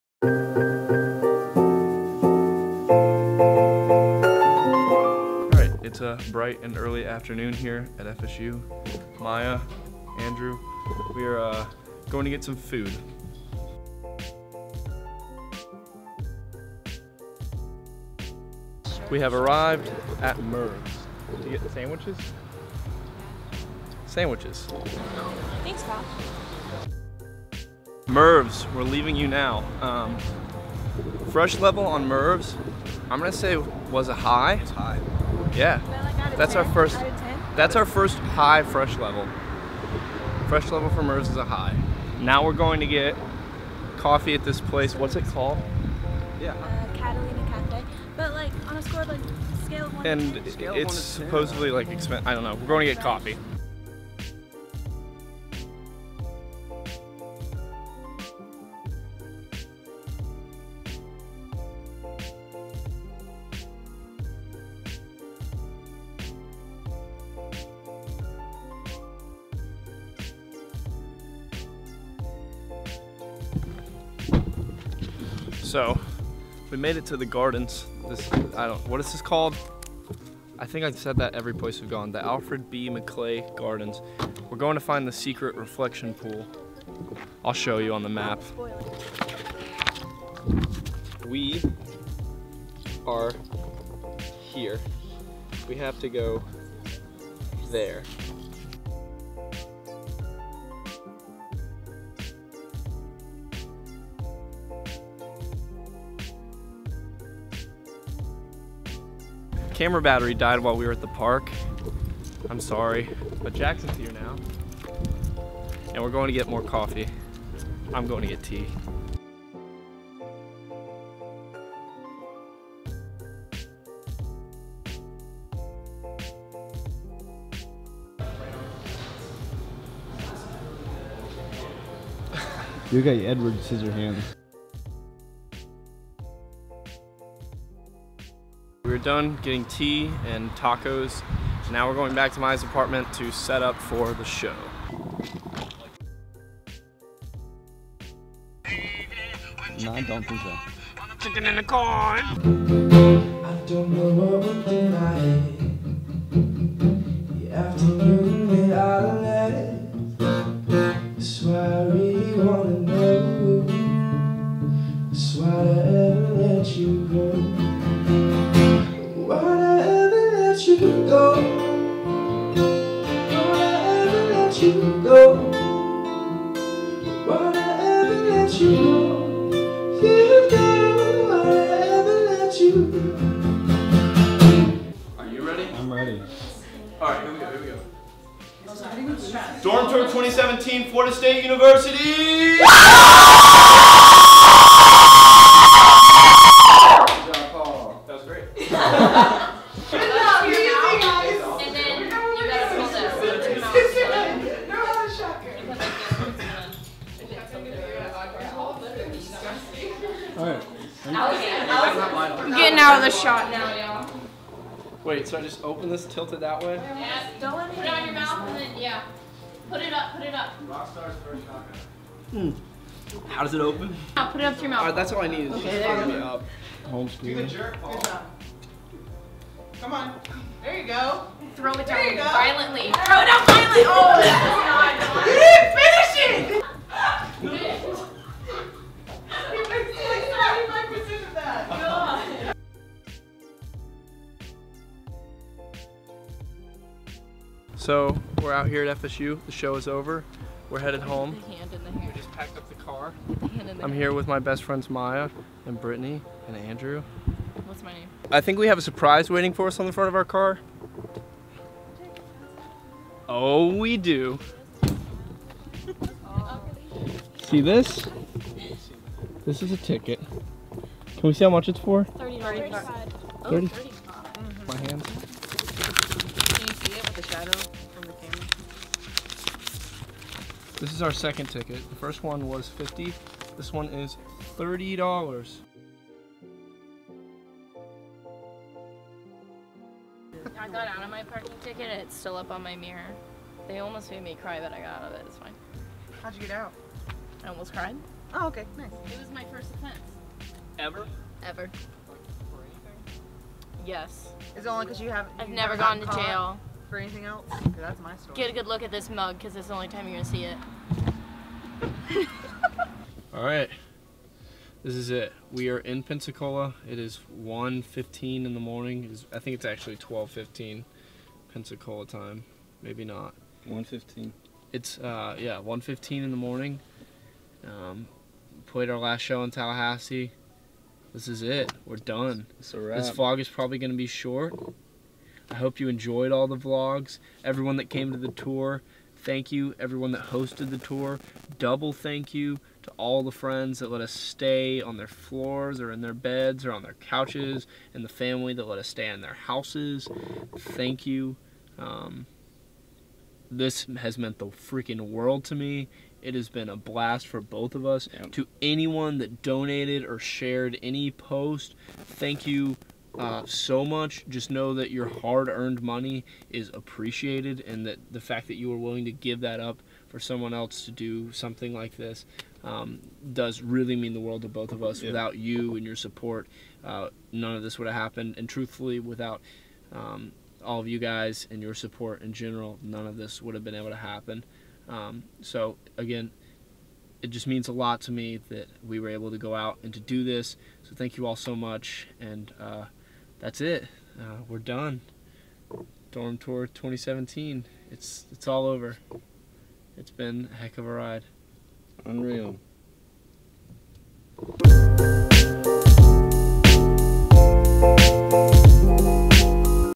DORM TOUR VLOG | Florida State University
This is it, the last vlog. I hope everyone has enjoyed these vlogs and don't forget to subscirbe if you did!